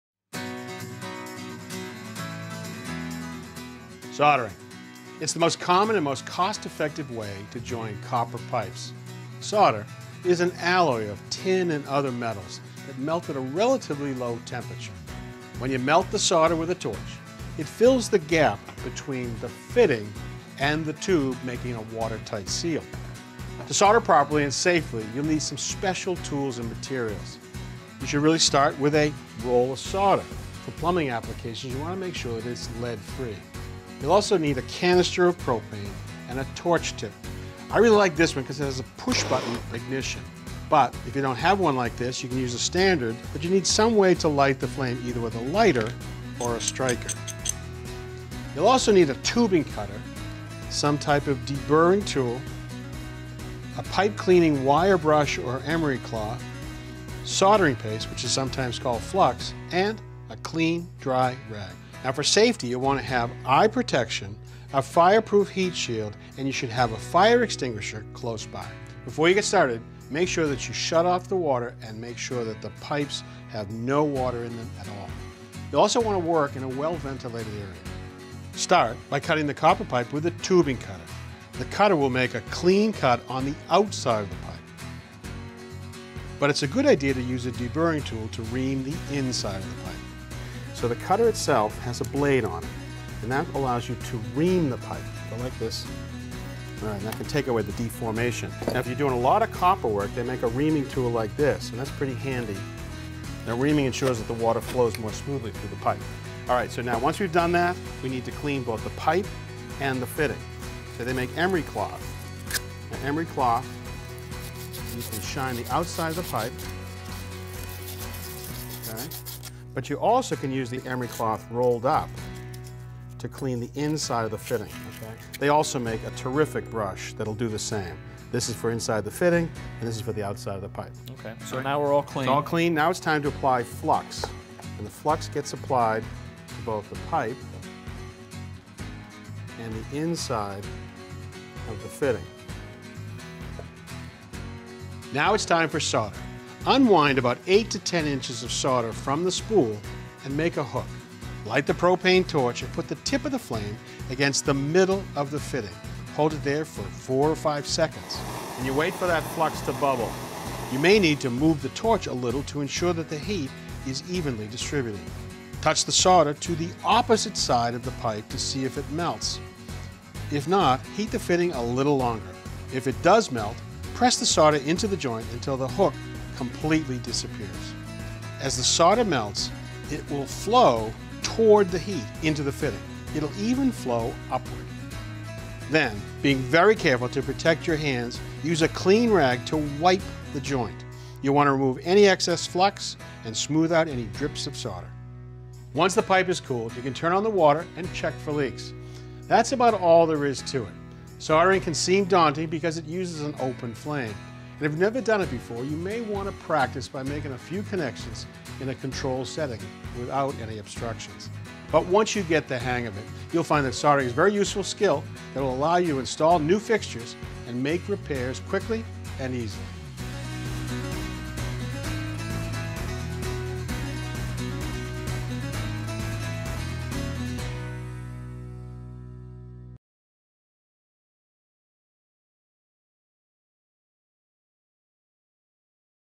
How To Solder Copper Pipes | The Home Depot
Learn how to solder copper pipe for DIY plumbing projects. Copper tubing is flexible, bacteria-resistant and easily installed. It’s also environmentally friendly since it can be recycled. Learn more about soldering copper pipes with our guide

This guide will show you how to solder copper pipes to ensure there are no leaks in or around your home. It also outlines options to solder without flame and to patch a pinhole with solder. The Home Depot will help you learn the basics of soldering copper pipe, including how to solder vertical copper pipe.